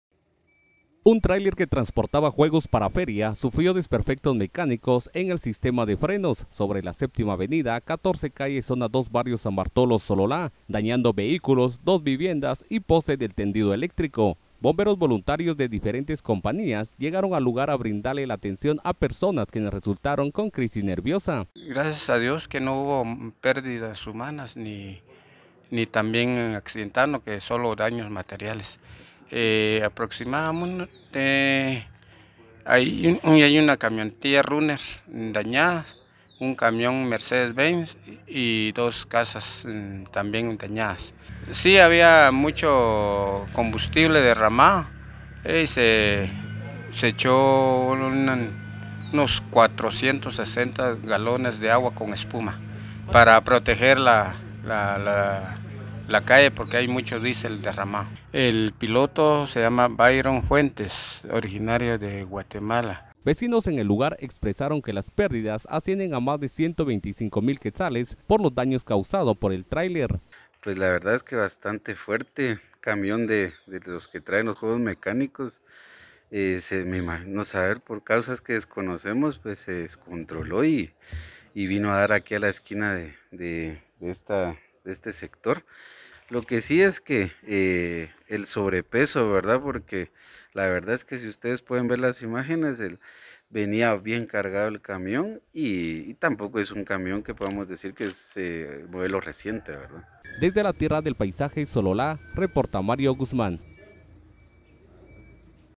Tráiler protagoniza accidente tras fallas en el sistema de frenos
Un tráiler que transportaba juegos para feria, sufrió desperfectos mecánicos sobre, 7.ª avenida y 14 calle de la zona 2, Barrio San Bartolo, Sololá. Terminó dañando vehículos, dos viviendas y dos postes del tendido eléctrico.